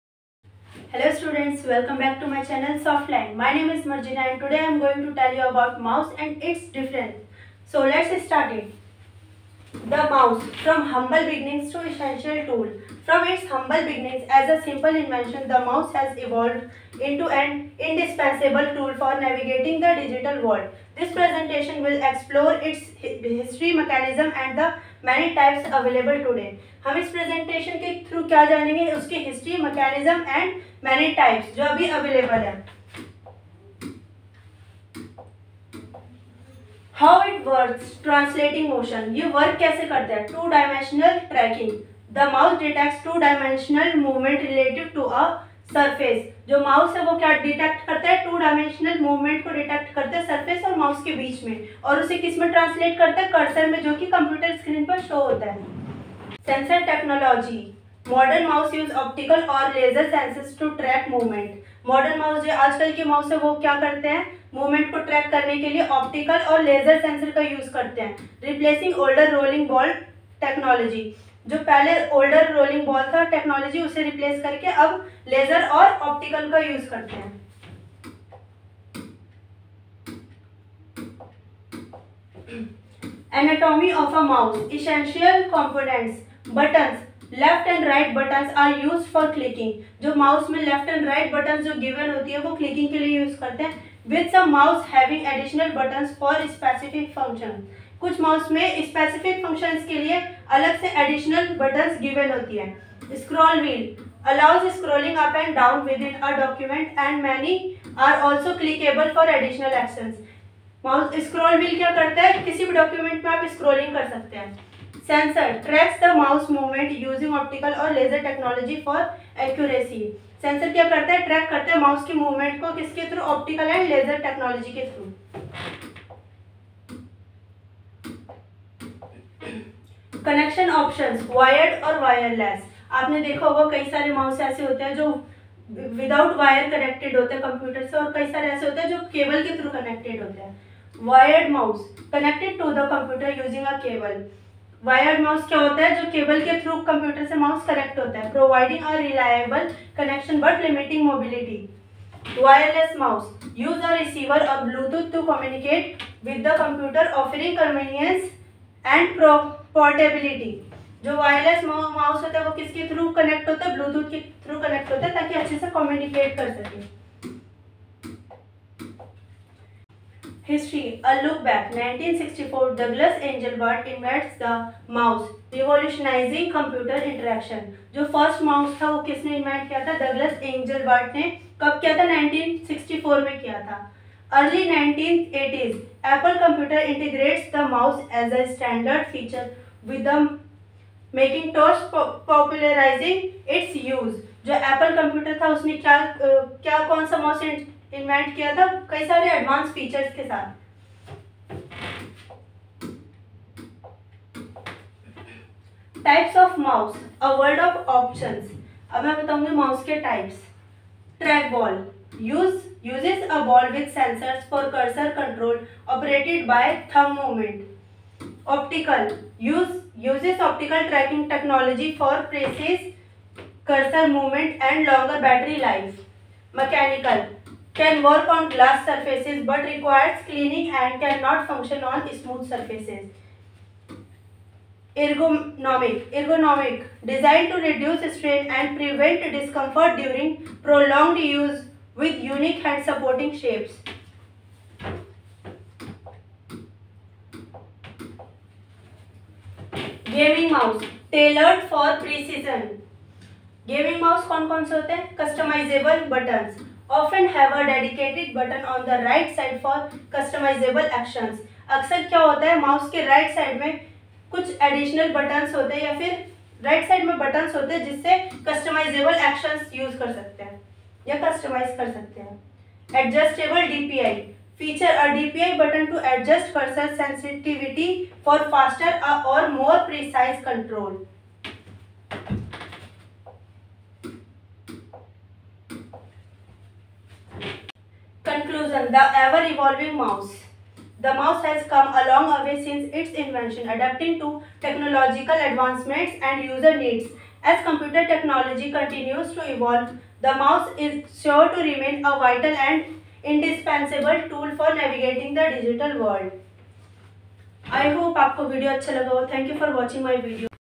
Understanding the Computer Mouse: Types, Functions & Features | Marjina Ma’am 🖱️
Dive into the basics of the computer mouse with Marjina Ma’am! 🖱️ Whether you're a beginner or a tech enthusiast, this video will guide you through the types of mice, their uses, and interesting features. Learn how this essential input device has evolved and why it remains crucial in the digital age.

🔍 What’s Inside the Video:
1️⃣ What is a computer mouse?
2️⃣ Different types: Optical, Mechanical, Wireless, and Trackball mice.
3️⃣ Functions and how it helps in navigation.
4️⃣ Tips and tricks for efficient use.
5️⃣ Fun facts about the evolution of the mouse!

👉 Join Marjina Ma’am in unraveling the secrets of this handy device! Perfect for computer beginners and enthusiasts alike.